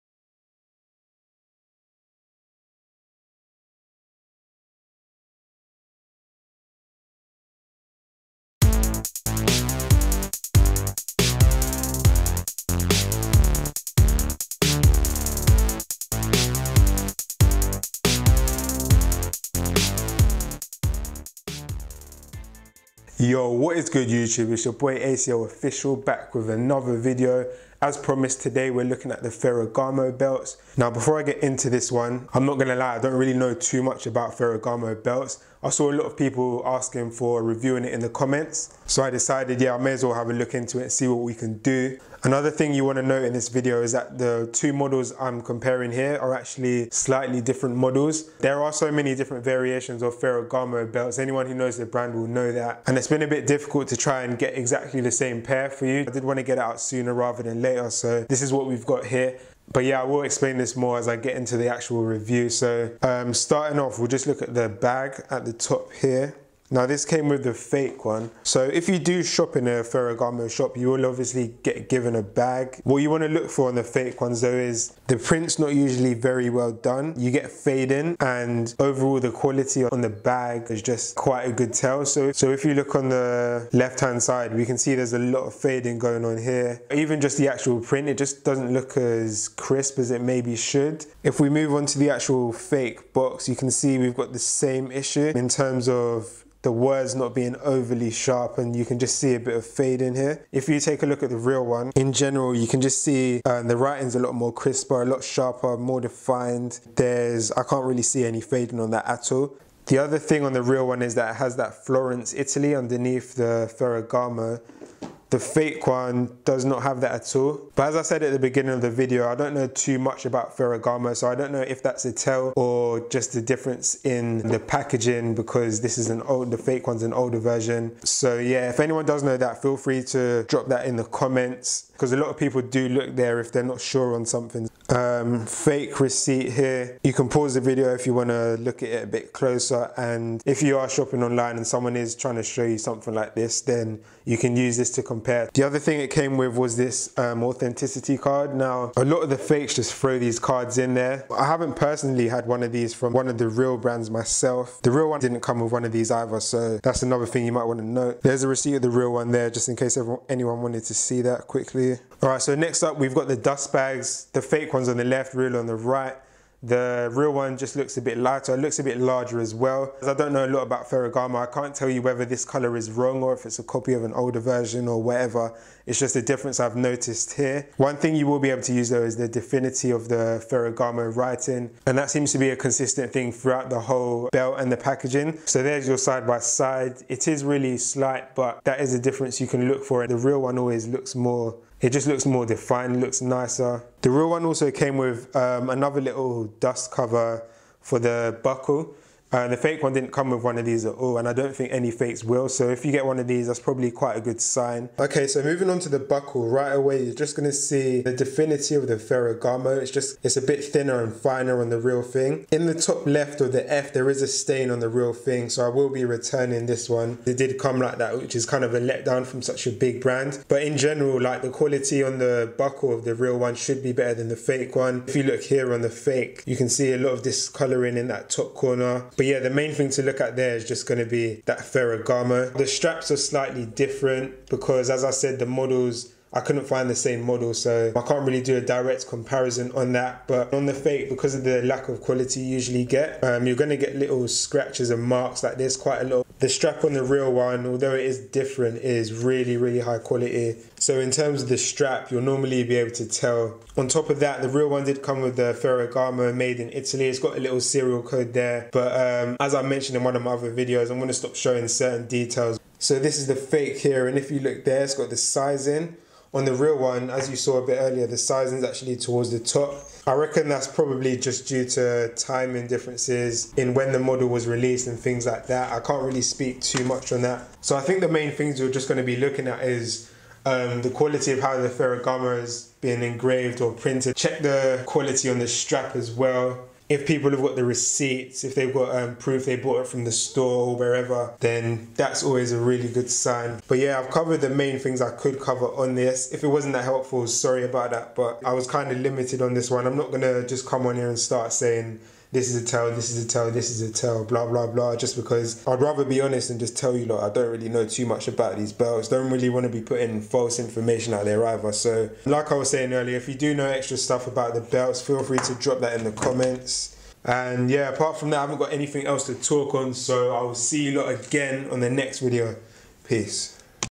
Real vs Fake Ferragamo Belt | How to spot fakes
REAL VS FAKE FERRAGAMO BELT. This real vs fake Ferragamo belt comparison attempts to give you some useful information in relation to checking the authenticity of Ferragamo belts. Ferragamo belts vary so by no means is this a wholly conclusive guide. Some of the differences described will not apply in all scenarios.

Ferragamo is no different to any of the other luxury brands in that its items are regularly replicated. The result of this is that many fakes begin to flood marketplaces which increases your risk of being scammed. Using a few of the tips given in this video will hopefully help you avoid that happening!

PREVIOUS VIDEOS